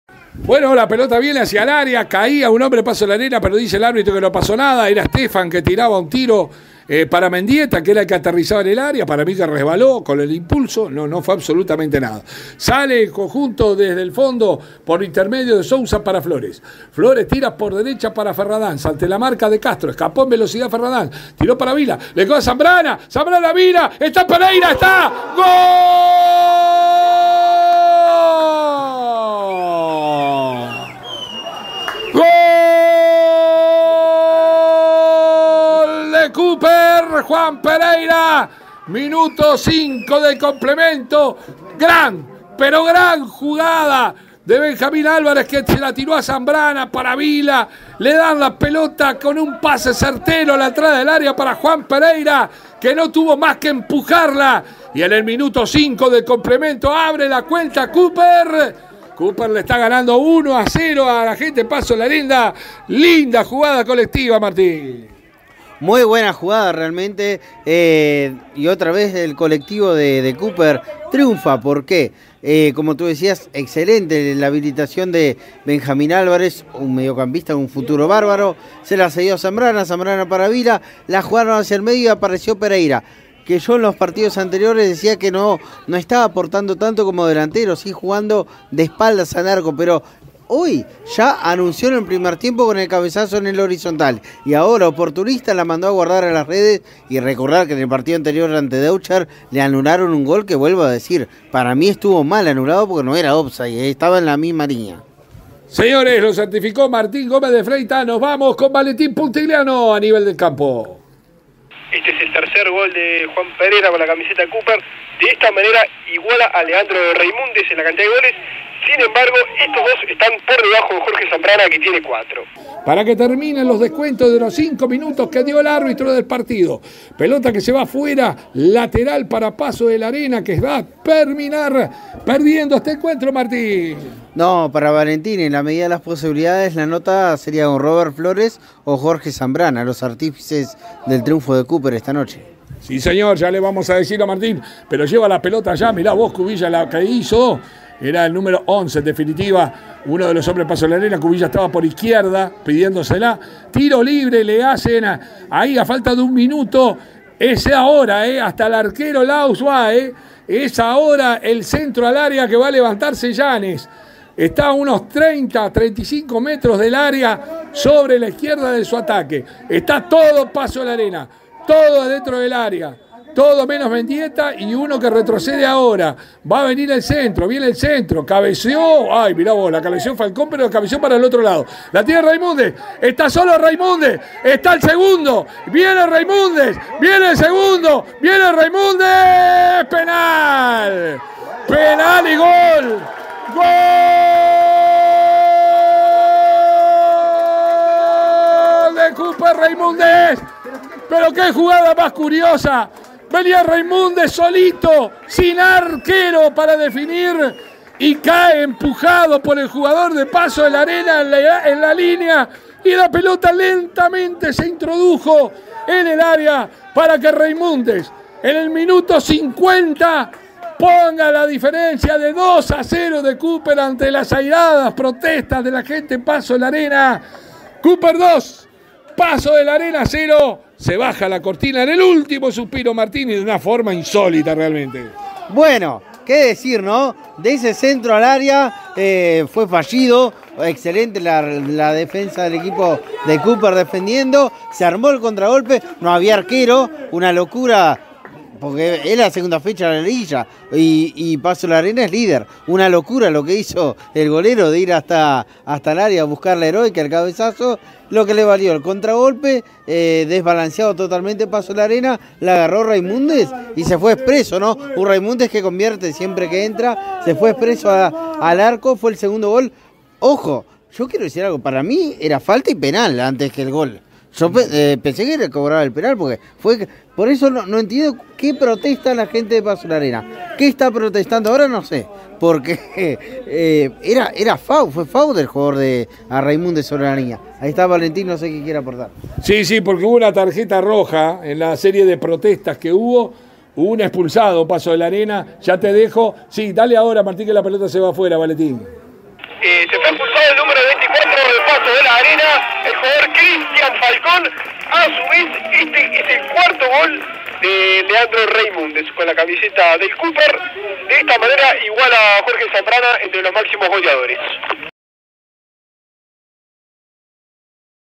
Cooper 2 Paso de la Arena 0 - Divisional D 2022 (por Alberto Avellaneda)
Los goles del partido, por Alberto Avellaneda, Martín Gómez de Freitas y Valentín Puntigliano para Día D Radio en Radio 4 Online.